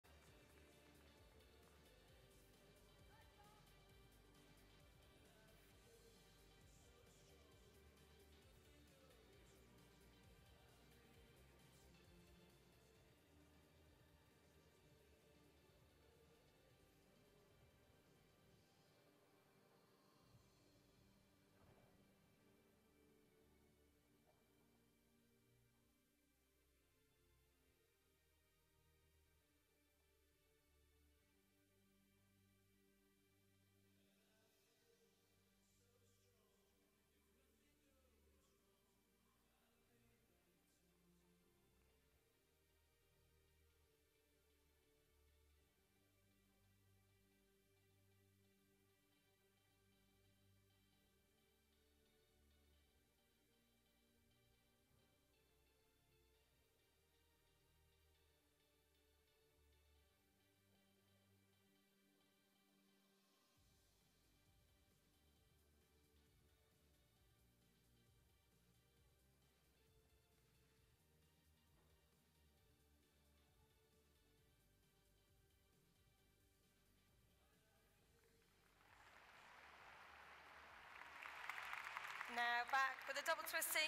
LOUISE MCCOLGAN - BEAM - 2013 British Champs - Apparatus Finals3
LOUISE BEAM routine at the 2013 Artistic Gymnastics British Championships.